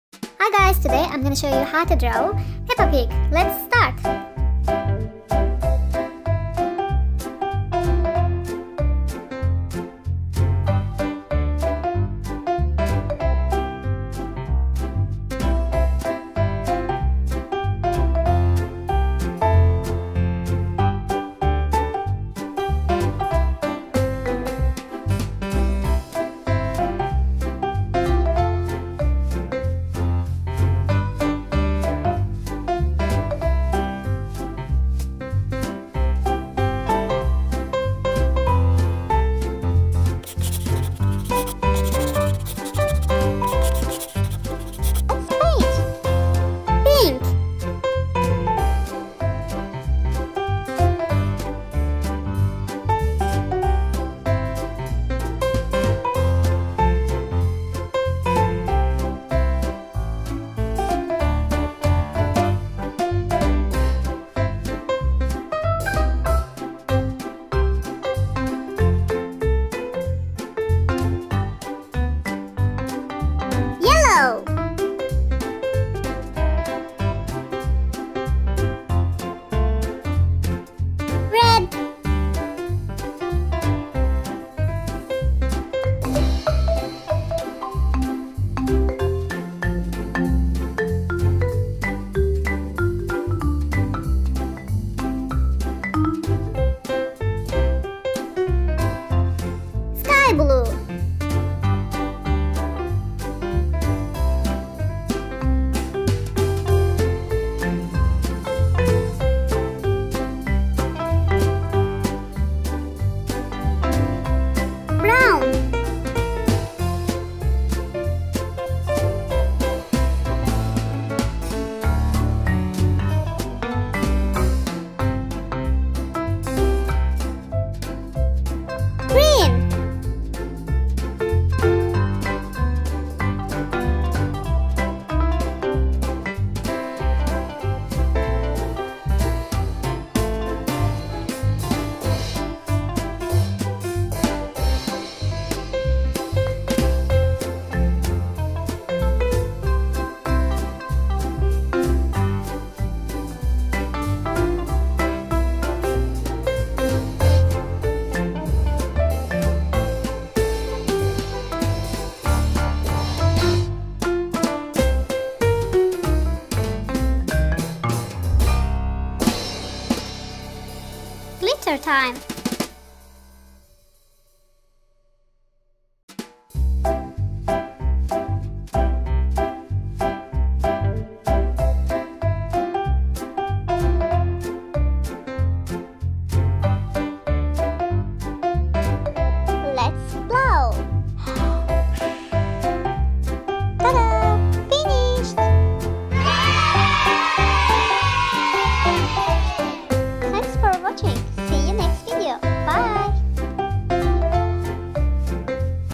Peppa Pig Puddle of Mud Drawing and Coloring | Painting Doodles for Kids
Hello!  Today we are learning how to draw Peppa pig jumping in a muddy puddle because Everything is better with doodles for kids!  Peppa lives with her mummy and daddy and her little brother, George. Her adventures are fun, sometimes involve a few tears, but always end happily.